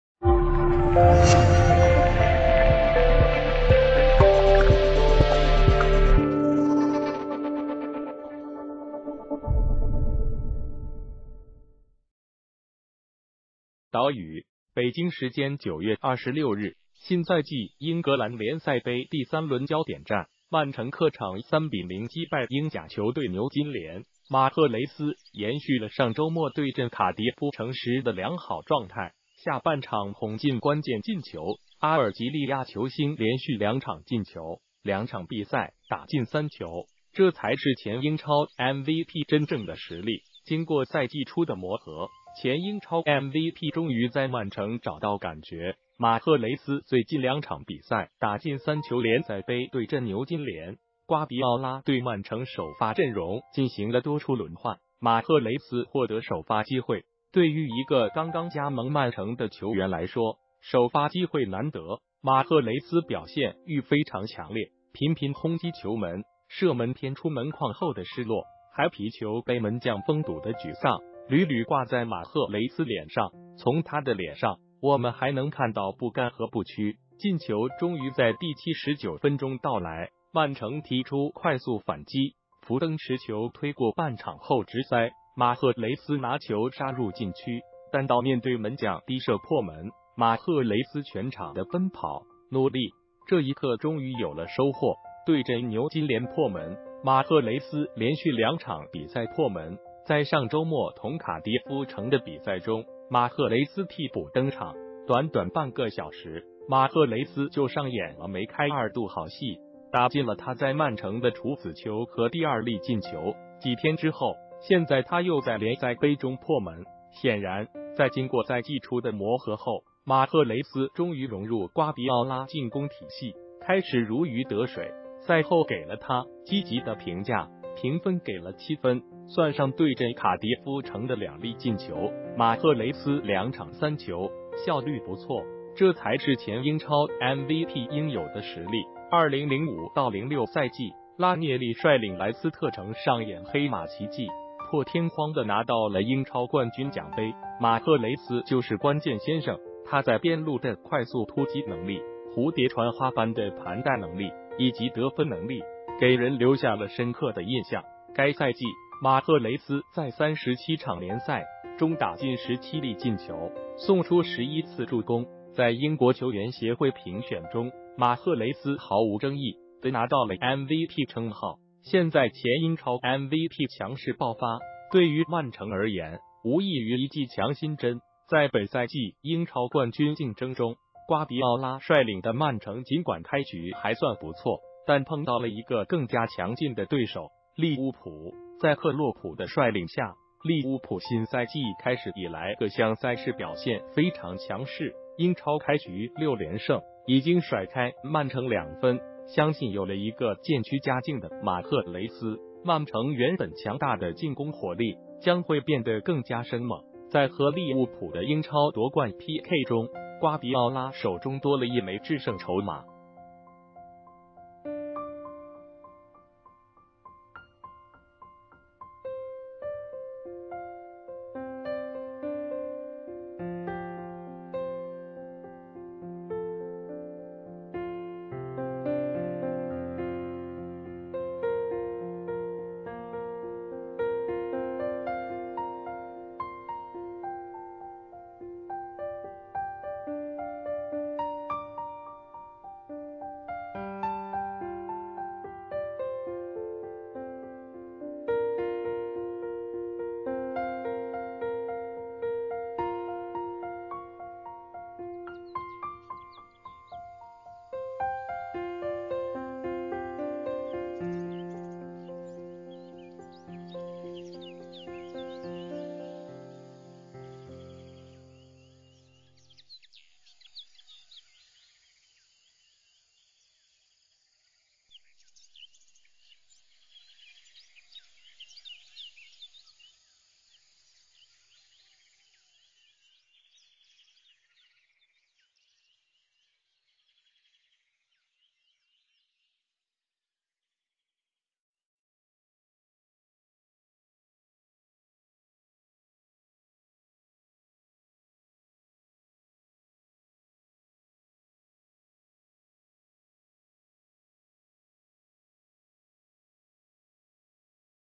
两场三球，马赫雷斯强势爆发，这才是前英超MVP应有水准
谢谢！导语：北京时间9月26日，新赛季英格兰联赛杯第3轮焦点战，曼城客场3-0击败英甲球队牛津联。马赫雷斯延续了上周末对阵卡迪夫城时的良好状态，下半场轰进关键进球。阿尔及利亚球星连续两场进球，两场比赛打进三球，这才是前英超MVP真正的实力。经过赛季初的磨合，前英超MVP终于在曼城找到感觉。马赫雷斯最近2场比赛打进3球联赛杯对阵牛津联，瓜迪奥拉对曼城首发阵容进行了多处轮换，马赫雷斯获得首发机会。对于一个刚刚加盟曼城的球员来说，首发机会难得，马赫雷斯表现欲非常强烈，频频轰击球门。射门偏出门框后的失落，还皮球被门将封堵的沮丧，屡屡挂在马赫雷斯脸上。从他的脸上，我们还能看到不甘和不屈。进球终于在第79分钟到来，曼城踢出快速反击，福登持球推过半场后直塞，马赫雷斯拿球杀入禁区，单刀面对门将低射破门。马赫雷斯全场的奔跑、努力，这一刻终于有了收获。对阵牛津联破门，马赫雷斯连续两场比赛破门。在上周末同卡迪夫城的比赛中，马赫雷斯替补登场。短短半个小时，马赫雷斯就上演了梅开二度好戏，打进了他在曼城的处子球和第二粒进球。几天之后，现在他又在联赛杯中破门。显然，在经过赛季初的磨合后，马赫雷斯终于融入瓜迪奥拉进攻体系，开始如鱼得水。赛后给了他积极的评价，评分给了7分。算上对阵卡迪夫城的两粒进球，马赫雷斯两场三球，效率不错，这才是前英超MVP应有的实力。2005/06赛季，拉涅利率领莱斯特城上演黑马奇迹，破天荒的拿到了英超冠军奖杯，马赫雷斯就是关键先生，他在边路的快速突击能力，蝴蝶穿花般的盘带能力，以及得分能力，给人留下了深刻的印象。该赛季，马赫雷斯在37场联赛中打进17粒进球，送出11次助攻。在英国球员协会评选中，马赫雷斯毫无争议的拿到了MVP称号。现在前英超MVP强势爆发，对于曼城而言，无异于一剂强心针。在本赛季英超冠军竞争中，瓜迪奥拉率领的曼城尽管开局还算不错，但碰到了一个更加强劲的对手，利物浦。在克洛普的率领下，利物浦新赛季开始以来各项赛事表现非常强势，英超开局六连胜，已经甩开曼城两分。相信有了一个渐趋佳境的马赫雷斯，曼城原本强大的进攻火力，将会变得更加生猛。在和利物浦的英超夺冠PK中，瓜迪奥拉手中多了一枚致胜筹码。